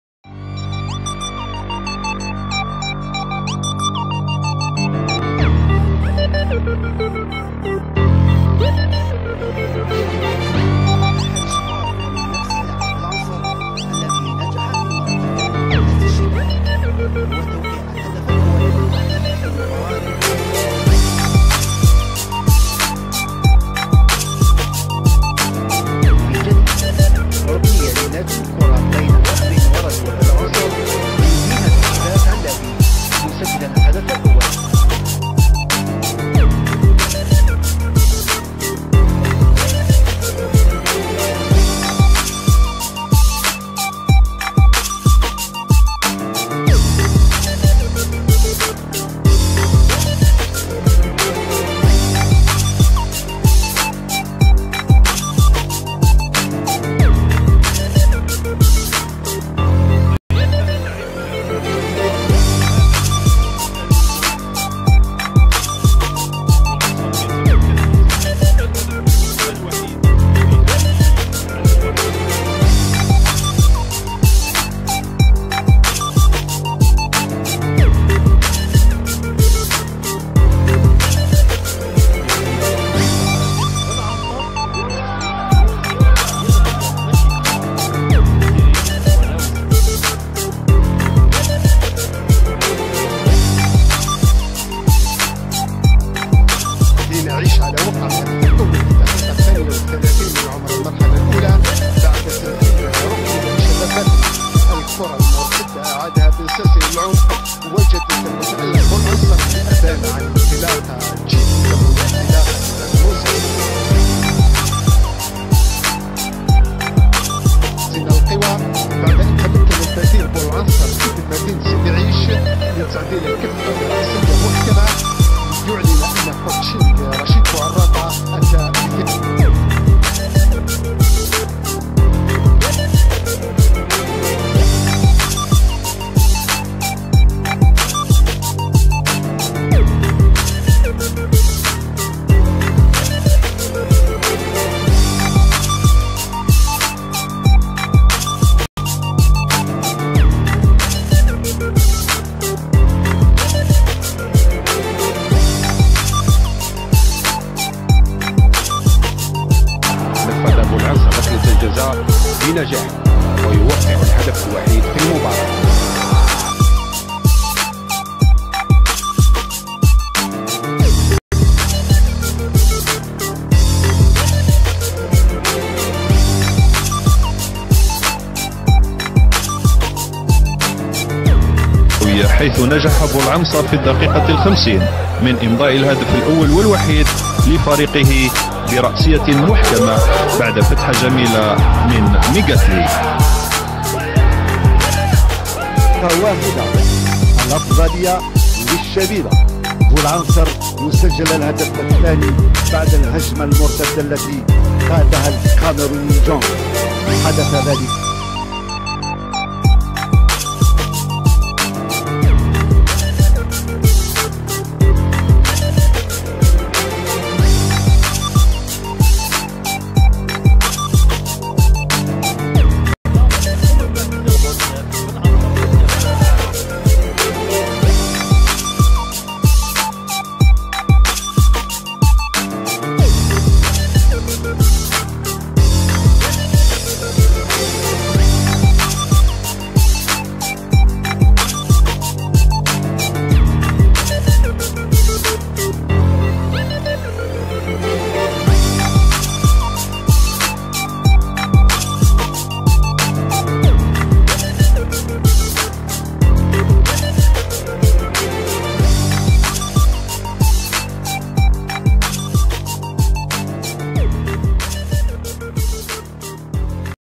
Rafik Boulanseur ● Goals, Assits, Dribbling Skills ● NC Magra 🔥
Rafik Boulanseur

Né le : 11 décembre 1991

Nationalité : Algérienne

Taille & Poids : 1m80 / 76kg

Poste : Attaquant de pointe

Polvalence : Attaquant de couloir

Club actuel : NC Magra

Club formateur : JSM Béjaïa

Ancien International avec les U23